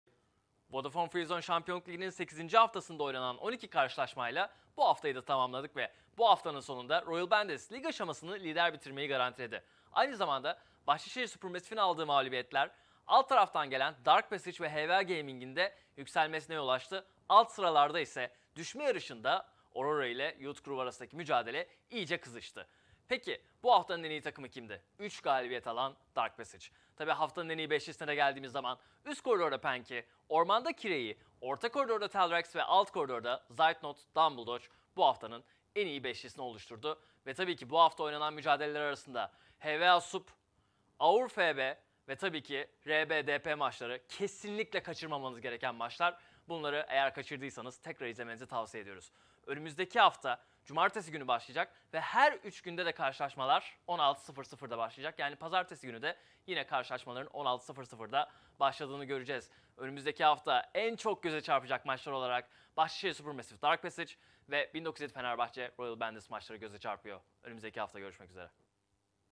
"Ton Balıklı Makarna" 8. Hafta'yı değerlendirdi! | VFŞL Yaz Mevsimi
Ligi lider bitirmeyi garantileyen takım kim? Hangi takımın aldığı mağlubiyetler DP ve HWA'nın yükselmesine neden oldu?

Cevaplar, "Ton Balıklı Makarna" ile VFŞL Yaz Mevsimi'nin 8. Hafta'sı özel değerlendirmesinde!